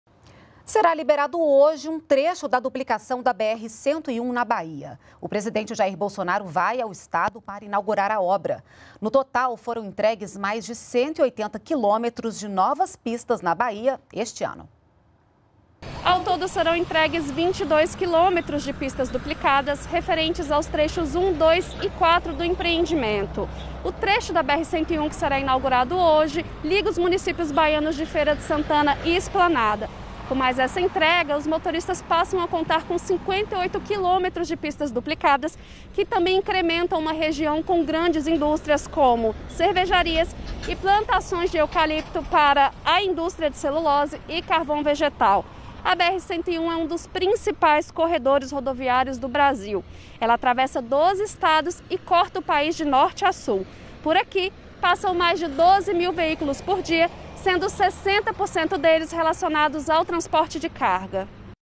Governo entrega 22 quilômetros de pistasduplicadas na Bahia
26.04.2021 - Será liberado hoje um trecho da duplicação da BR-101, na Bahia. O presidente Jair Bolsonaro vai ao estado para inaugurar a obra.